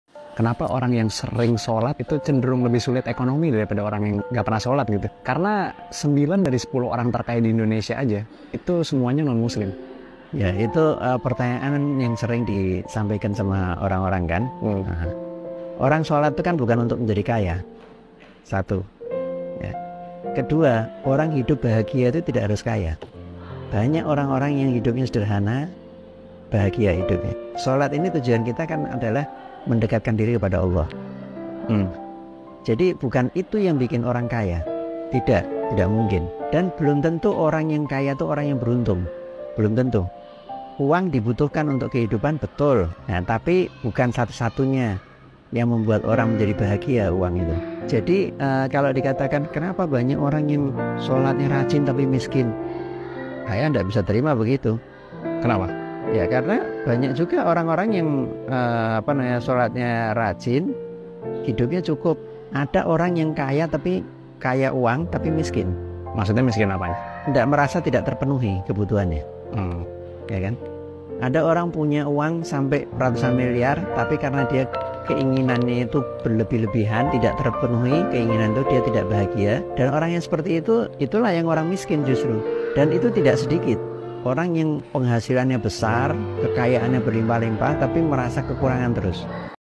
BANYAK UANG TAPI HIDUPNYA MISKIN
Banyak Orang-orang hebat yang mempunyai banyak Uang tapi hidupnya miskin,